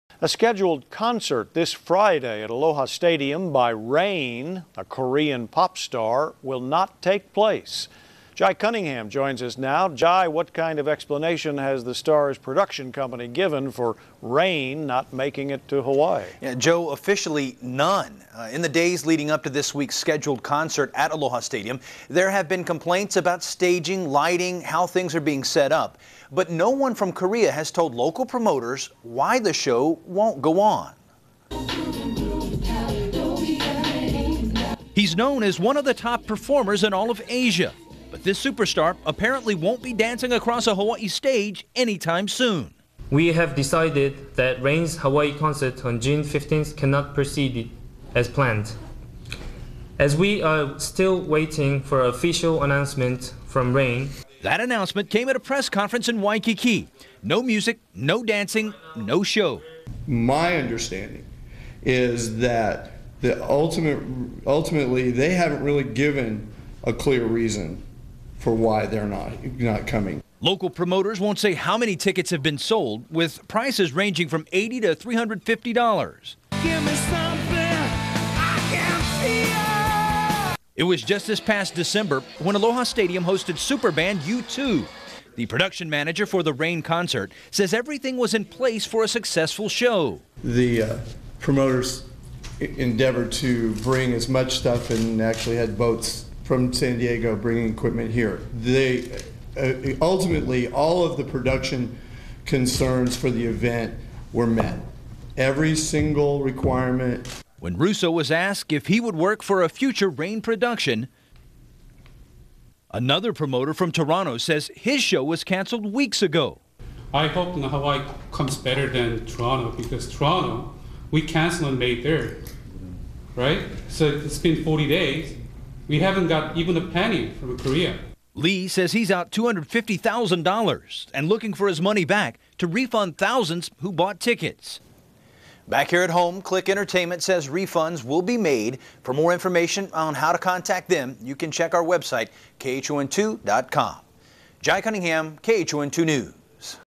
Bi/Rain Hawaii Concert Cancelled (KHON)
Local station KHON2's news report from 06/12/07 on the press conference announcing the cancellation of Bi/Rain's concert that was to take place on Friday 06/15/07 at Aloha Stadium.